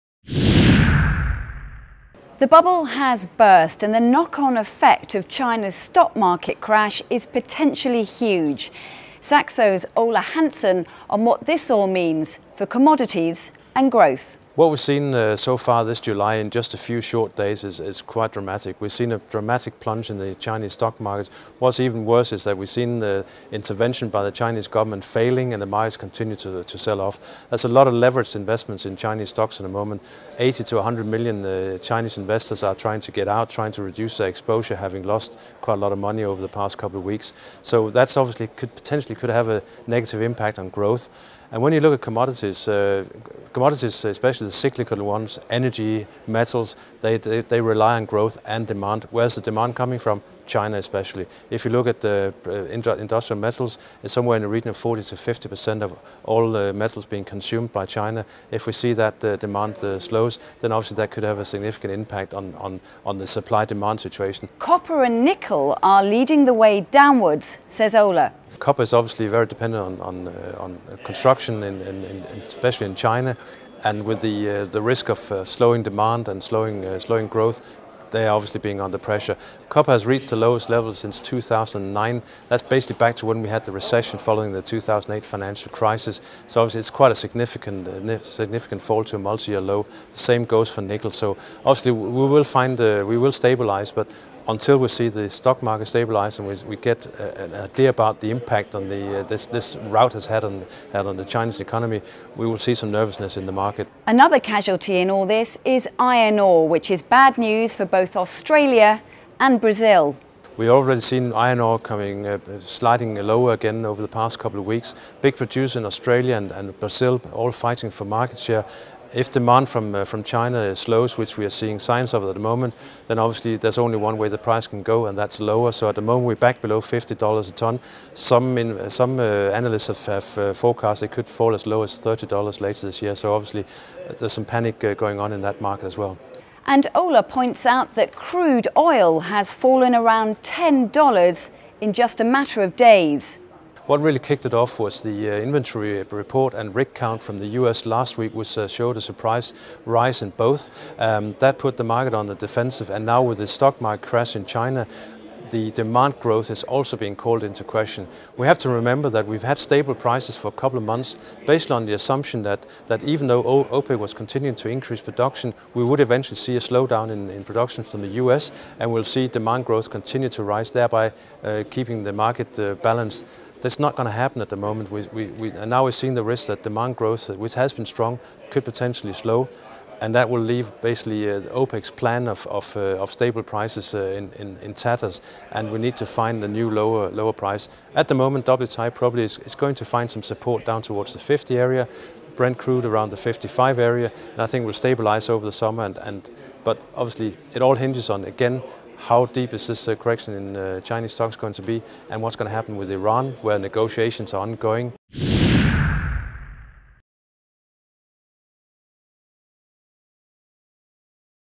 Metals melt, prices plunge as China crisis hits commodities
The Chinese stock market bubble has burst and so far, despite Chinese
state intervention, there looks to be no end in sight. Saxo's Ole
Hansen says 80 to 100 million Chinese investors have started to panic
and are trying to reduce their exposure. Commodities - in particular
cyclical ones such as energy and metals - are being hit hard.
China consumes 40-50% of all industrial metals. Hansen says that
copper and nickel are leading the sell-off with copper reaching its
lowest level since 2009. Iron ore has also been hit, which is bad news
for Australia and Brazil.
Crude oil has fallen around USD 10 in just a matter of days. Ole says
the slide began after last week's US inventory report, which put the
market on the defensive. The stock market crash in China has
exacerbated the situation. He believes WTI will find support around
the USD 50 a barrel level and USD 55 for Brent. Eyes are also on the
talks with Iran which have yet again been postponed.